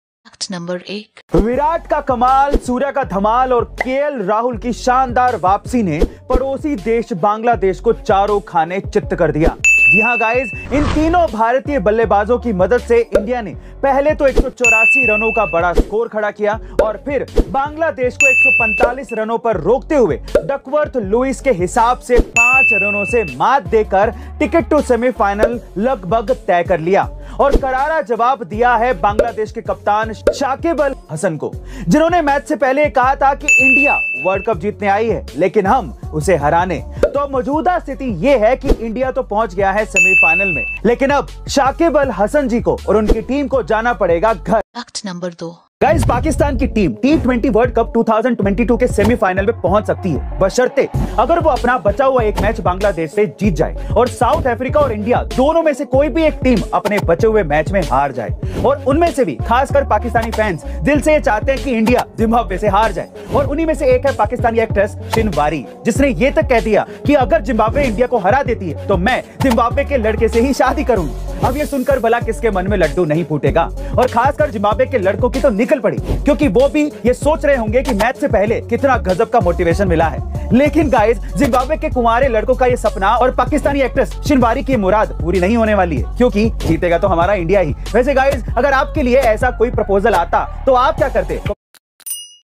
Pakistani लड़की फंसी, किसके साथ करेंगी शादी 🥰 Pakistan सेमीफाइनल में ! Ind vs Zim
समीकरण बदल गया है जो पाकिस्तानी मोहतरमा जिम्बाब्वे के व्यक्ति से विवाह करने वाली थी उसे अब डकवर्थ लुईस नियम के अनुसार नीदरलैंड के व्यक्ति से विवाह करनी पड़ेगी... 😂😂😂

If You Like This Video So Do Not Forget

ind vs eng final
t20 world cup match
t20 world cup match hilight
a2sir a2 knowledge
a2
A2Sir